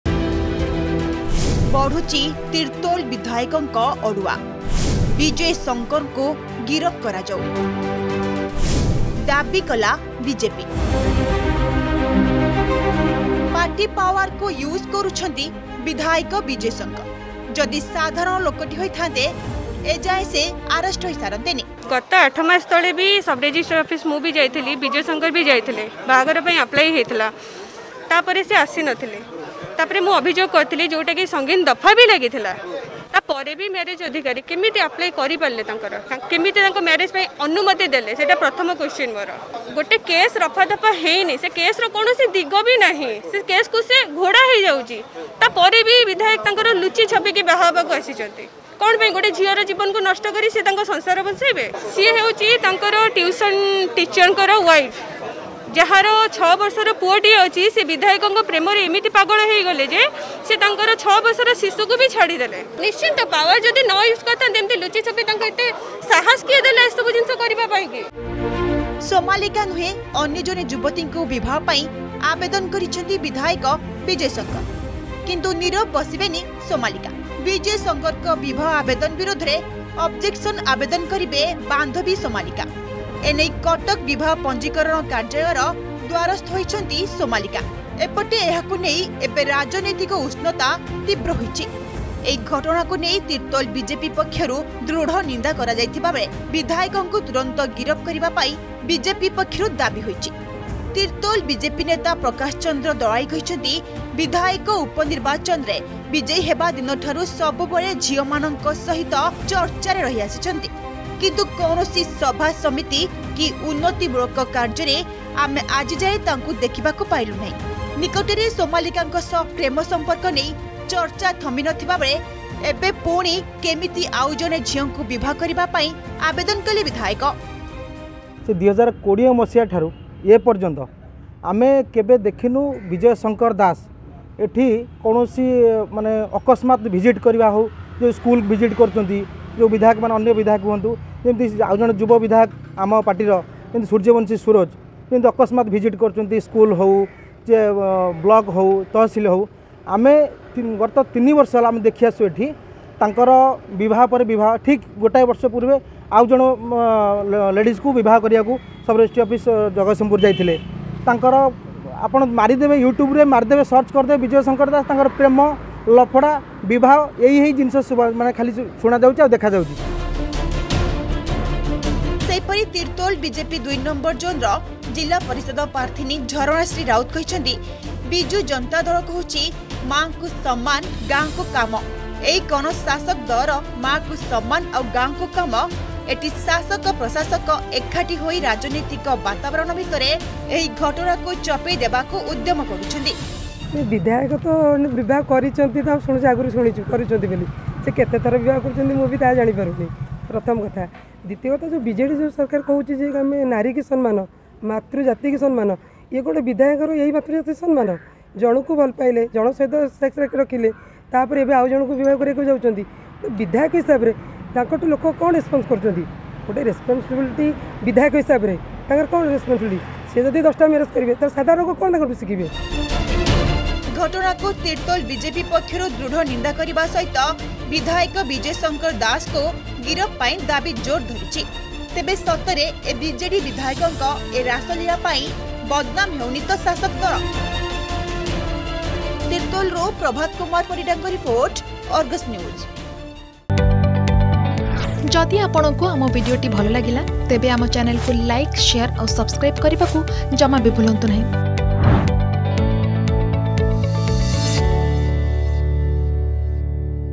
Problem Mounts For Tirtol MLA Bijay Shankar Das!
Trouble mounts for Odisha MLA as estranged girlfriend files complaint against marriage registration application.